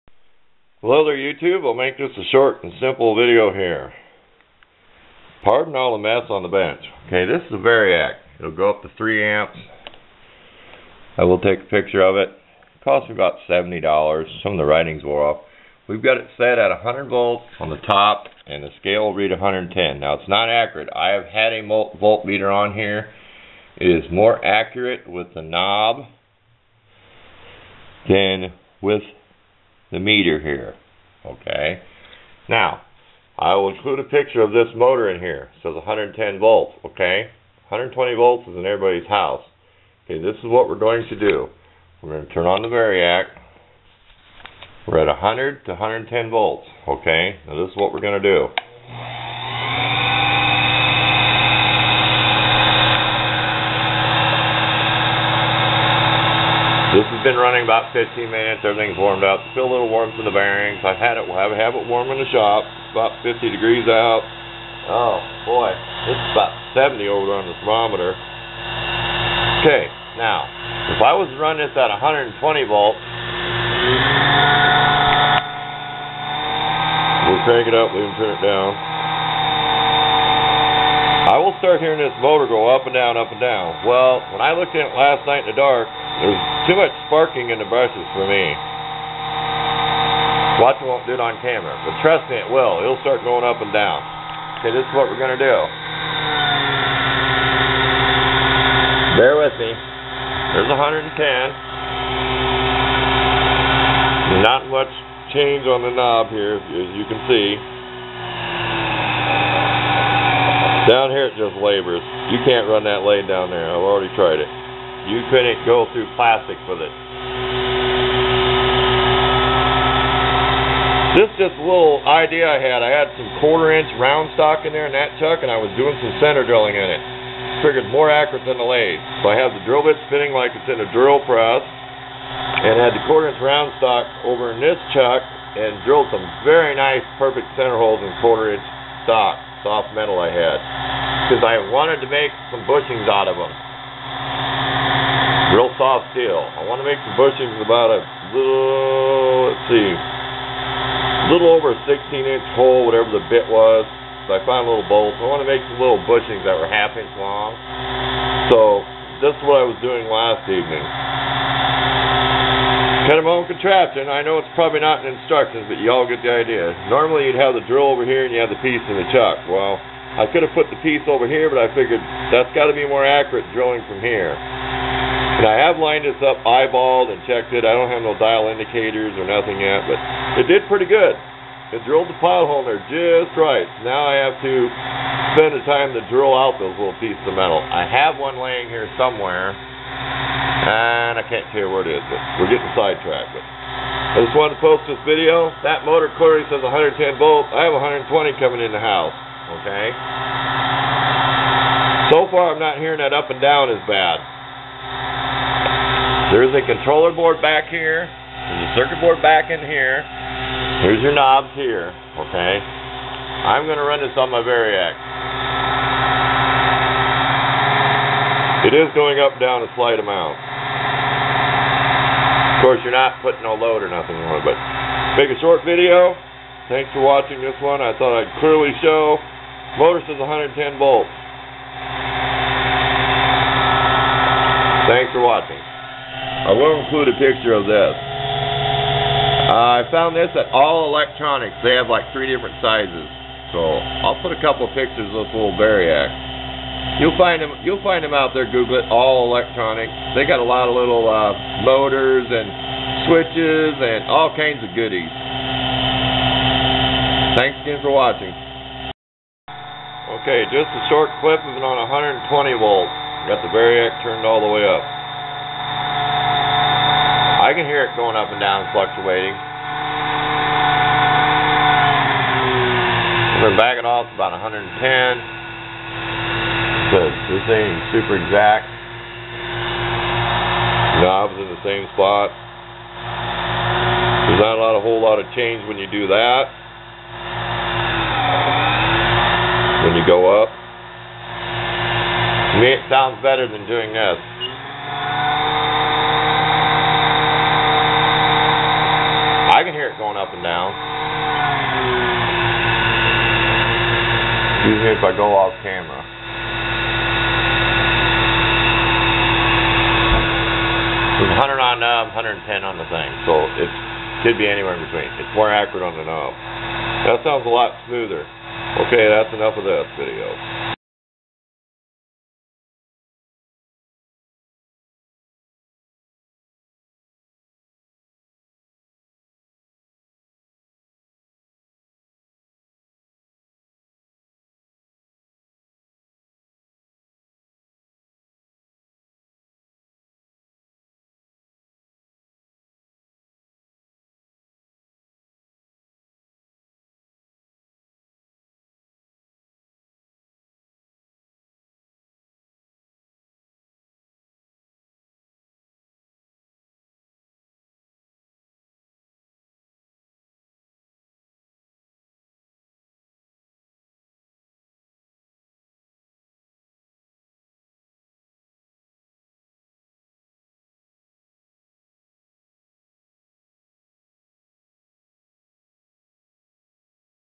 Nano Lathe Motor ( Says on tag 110 volts )
The motor tag on this Lathe says 110 Volts I know that it is running through a controller- circuit board I am not an Expert in Electronics But it runs smoother through my Variac The speed control on the lathe does not show alot of differeence from half way to full speed when using the Variac But without it Full speed on the Lathe knob does not give it a much faster speed any way This seems to run smoother through the Variac TRY THIS AT YOUR OWN RISK !!! I am just trying not to have another motor Failure NOTE I have a video made of how much the motor brushes Spark on 120 volts and How much less they Spark using the Variac on 100-110 Volts  I will post it soon